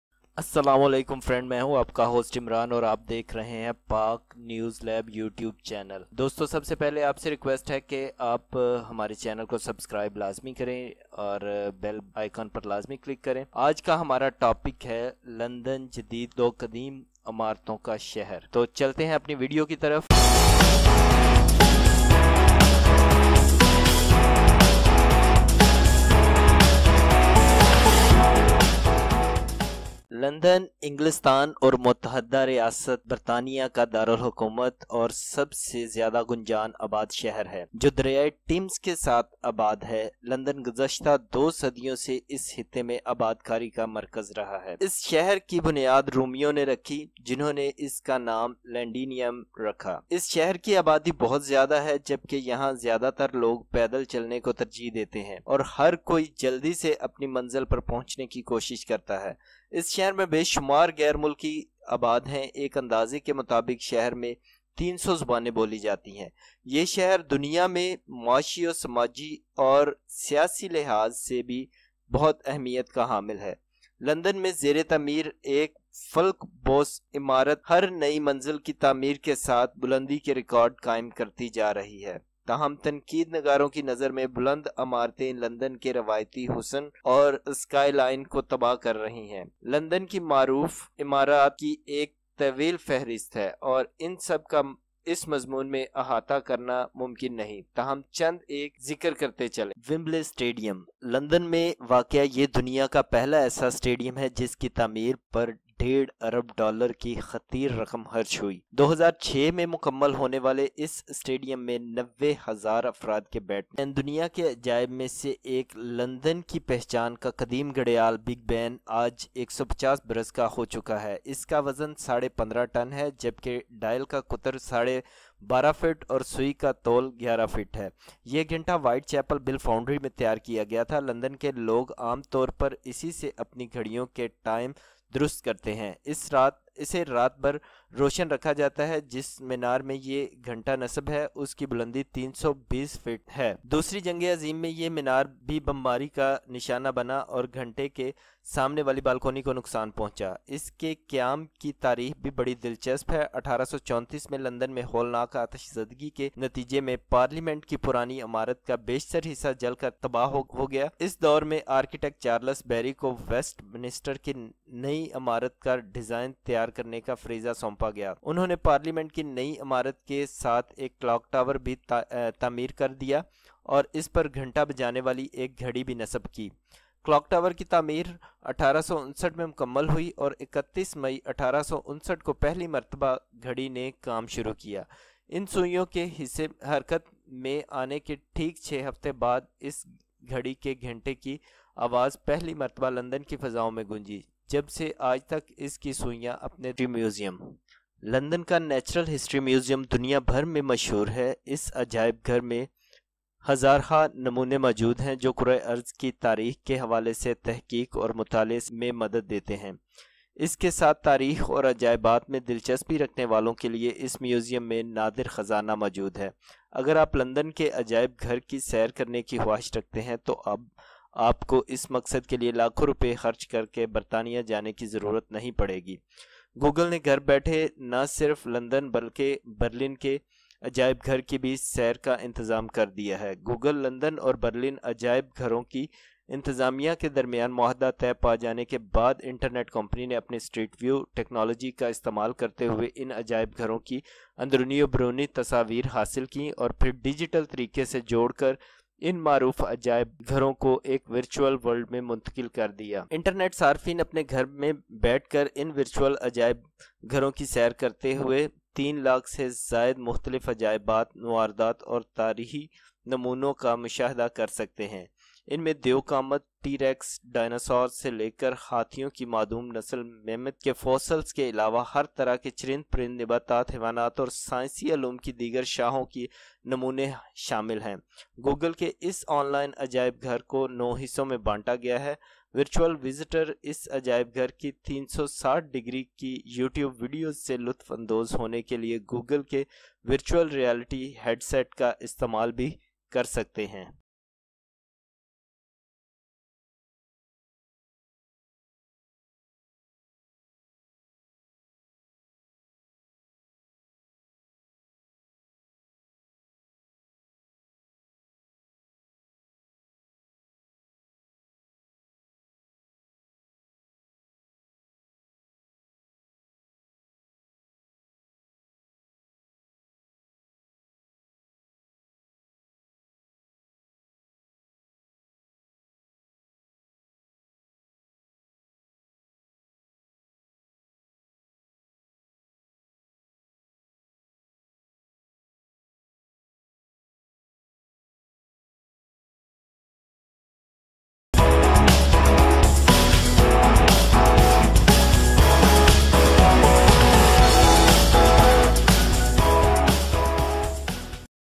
The London City A beautiful blend of Ancient and Modern buildings
London is the capital and most densely populated city in the United  Kingdom, along the River Thames. London has been a center of settlement in the region for the past two centuries. The city was founded by the Romans, who named it Lindenium. The population of this city is very high while most of the people here prefer to walk and everyone tries to reach their destination quickly.

The city is home to many foreigners. An estimated 300 languages ​​are spoken in the city. The city is also important in the world economically, socially and politically. A skyscraper under construction in London is setting a record for height with each new floor being built, but critics see tall buildings destroying London's traditional beauty and skyline. There is a long list of famous buildings in London and it is not possible to cover them all in this article, but let's mention a few.

London is a city of modern and ancient buildings
Wembley Stadium

Located in London, it is the first stadium in the world to cost a whopping 1.5 1.5 billion to build. Completed in 2006, the stadium can seat 90,000 people.

Big Ben

One of the wonders of the world, London's world-famous ancient crocodile "Big Ben" is 150 years old today. It weighs fifteen and a half tons, while the diameter of the dial is twelve and a half feet and the length of the needle is eleven feet. The watch was made at the White Chapel Bill Foundry. This is how Londoners usually adjust their clocks. It is kept on all night. The tower in which the clock is installed is 320 feet high. The tower was also bombed during World War II and damaged the balcony in front of the clock.

The date of its founding is also very interesting. In 1834, a terrible fire in London destroyed most of the old Parliament building, during which time the architect Charles Berry was commissioned to design the new Westminster building. went. He also built a clock tower next to the new parliament building and installed a clock on it. The clock tower was completed in 1859, and the clock began work for the first time on May 31, 1859.

Exactly six weeks after the needles began to move, the sound of the clock's echo resounded in London for the first time. Ever since, its needles have been spinning in its circle.

Museum of Natural History

London's Natural History Museum is world famous. The museum has thousands of specimens, which help in research and study of the history of the planet. In addition, for those interested in history and wonders, this museum has a rare treasure. If you want to visit the London Museum, you will no longer have to spend millions of rupees to go to the UK for this purpose.

Google has arranged a tour of museums not only in London but also in Berlin. After an agreement was reached between Google, the London and Berlin museums, the Internet company used its "Street View" technology to capture the interior and exterior images of these museums and then digitally combine them into well-known ones. Moved museums into a virtual world.

Internet users can visit these virtual museums at home and view more than 300,000 different museums, artefacts and historical artifacts. They range from giant T-rex dinosaurs to extinct elephant mammoth fossils, as well as specimens from all kinds of animals, birds, plants, animals and other branches of science. Google's online museum is divided into nine sections. Virtual visitors can also use Google's virtual reality headset to enjoy 360-degree YouTube videos from the museum.

St. Paul's Cathedral

London is a 2,000 year old city. In 1666, when a fire engulfed the whole of London, it was considered a mercy because it had ended the plague. A memorial tower with 311 steps has been erected at the site of the fire. Prominent philosophers and scientists Darwin and Newton are buried in Westminster.

Many famous painters, architects and scientists are buried in St. Paul's Cathedral. St. Paul's Cathedral has more than 500 steps and overlooks the entire city. The late Lady Diana and Prince Charles were also married here.